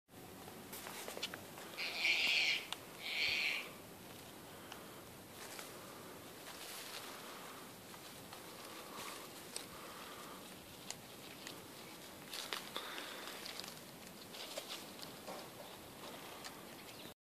Kaka feeding on Stewart Island
Kaka - Nestor meridionalis, feeding on the rare plant the Kaka Beak - Clianthus puniceus.